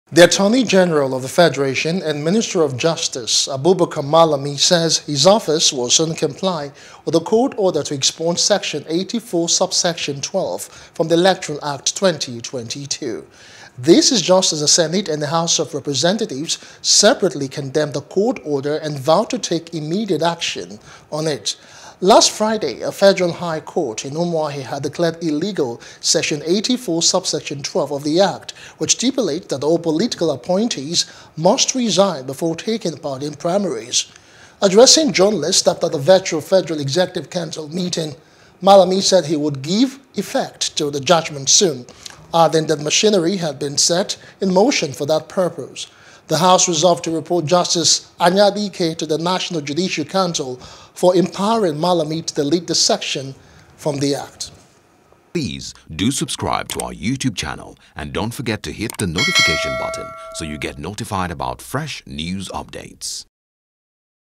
Electoral Act: NASS Plans Appeal, Tackles Judge, Malami Insists On Amendment | NEWS
The Attorney-General of the Federation and Minister of Justice, Abubakar Malami says his office will soon comply with the court order to expunge Section 84 (12) from the Electoral Act, 2022.